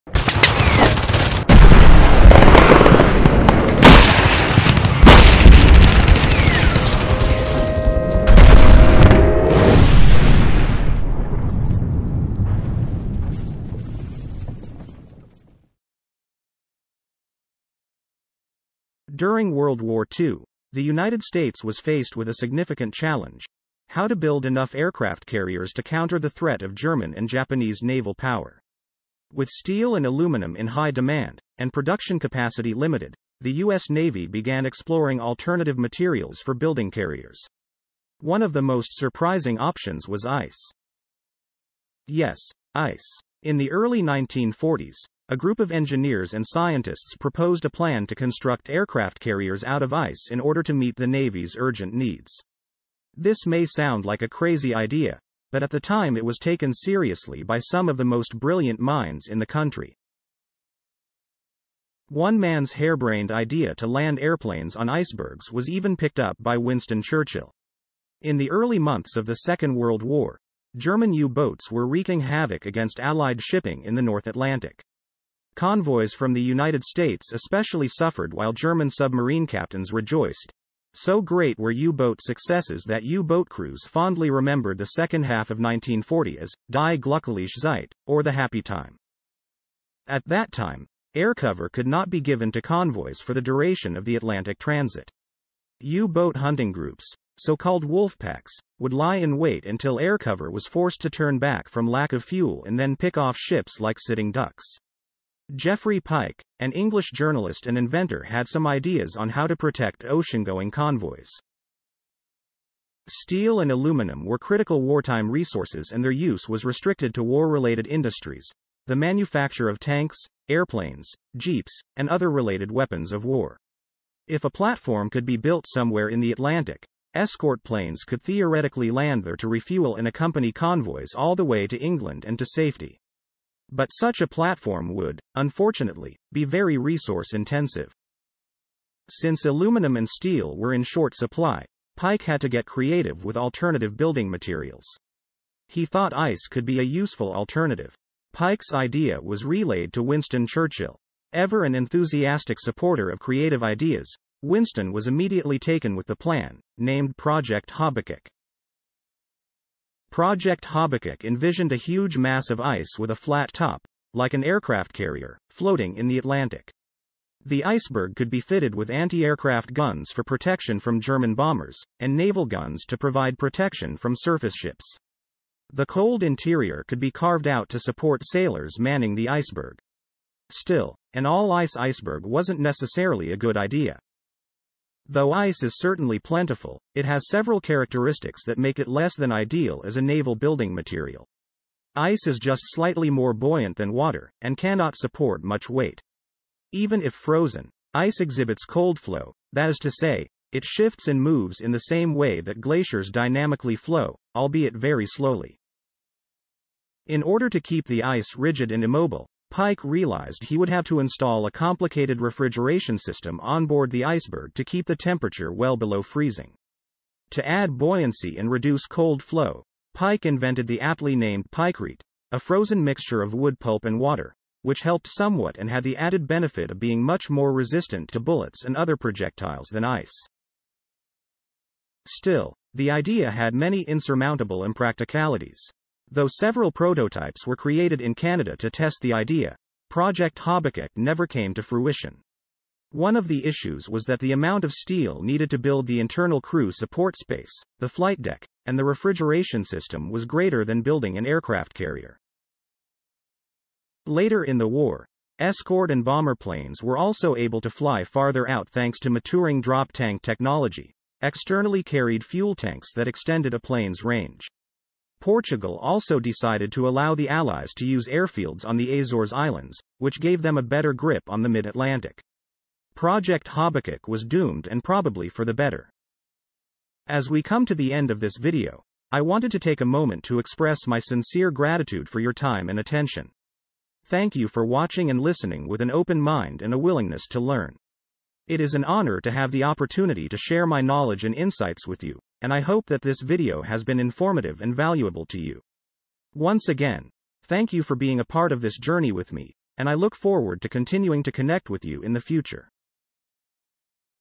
The Frozen World War II Plan to Build Aircraft Carriers Out of Ice
The World War II plan to build aircraft carriers out of ice was a bold and audacious strategy that has largely been forgotten by history. In this video, we dive into the fascinating story behind this plan and explore how it could have changed the outcome of the war. From the challenges of constructing an ice-based carrier to the potential advantages it could have offered, this video provides a comprehensive look at this little-known aspect of World War II history.